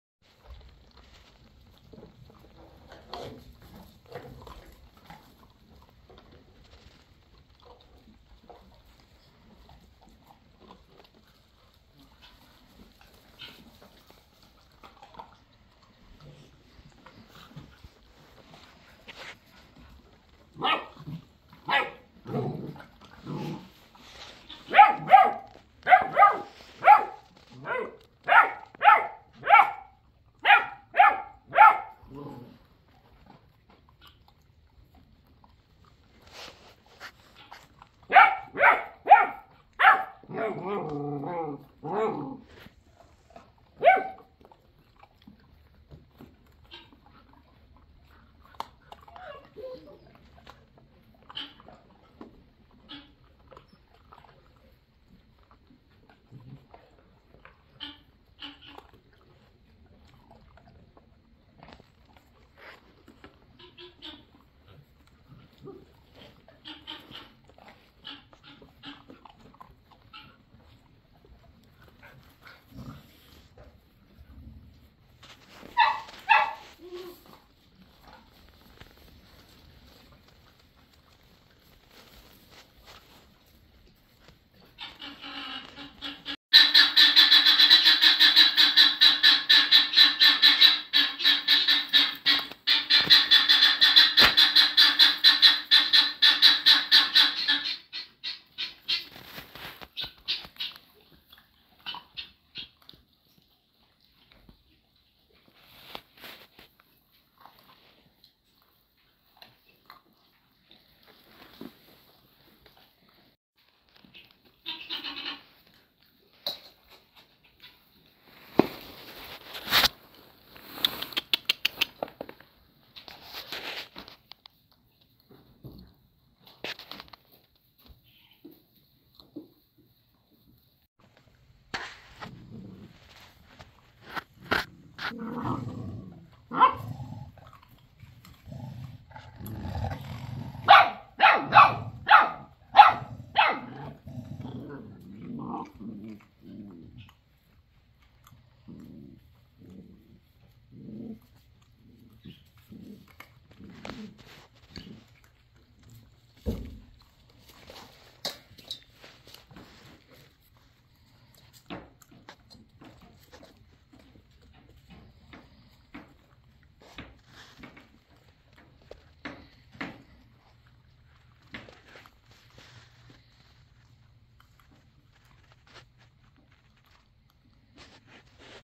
Casual afternoon in the Barn at feeding time
this is how I spend my time during afternoon feedings! the puppies are HUGE!